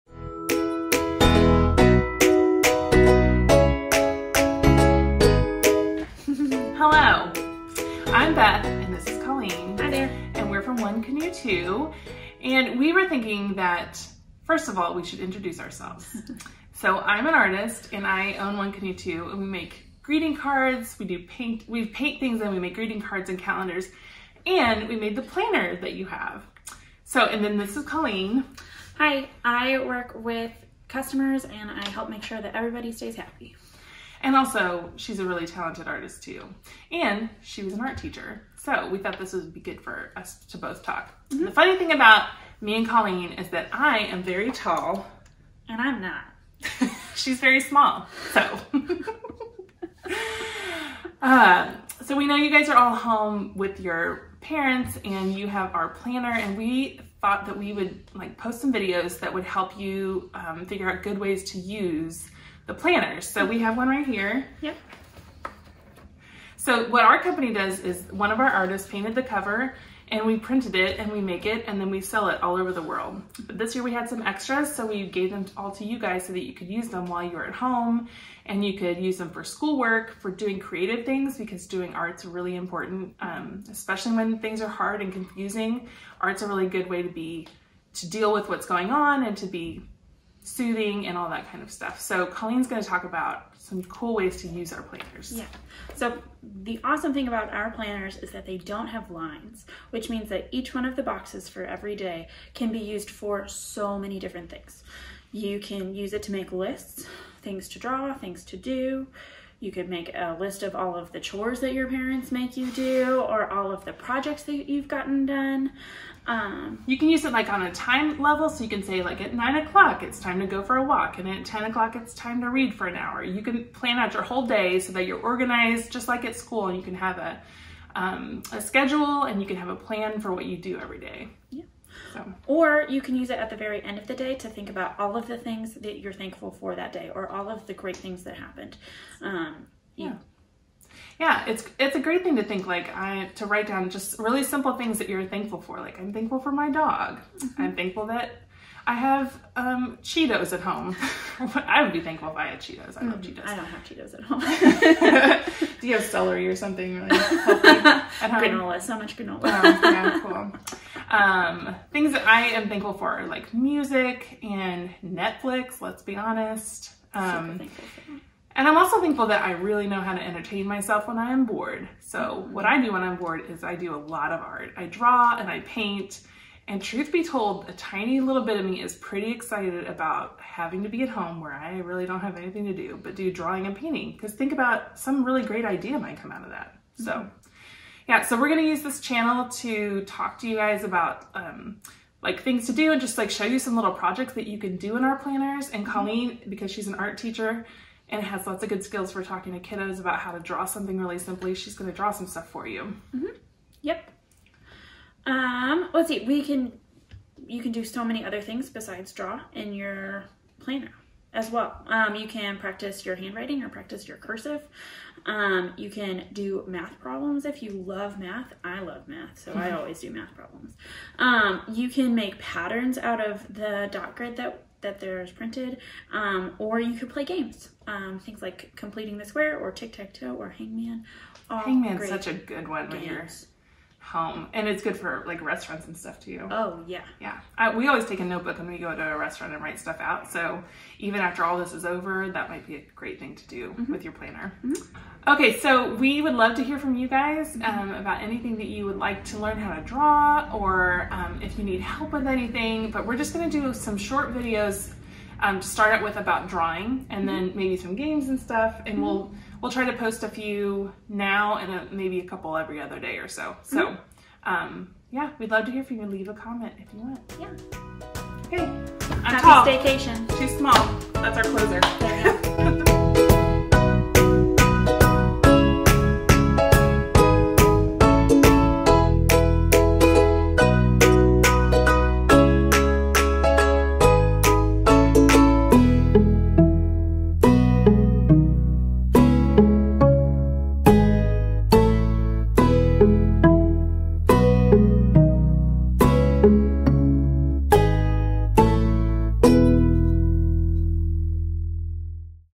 1c2 Kids Lesson 1: What to do with your planner!
We want everyone to use their planner and have so much fun! We'll be teaching you how to draw some fun things, make lists, journal and more! Leave us a comment about what you'd like to learn how to draw!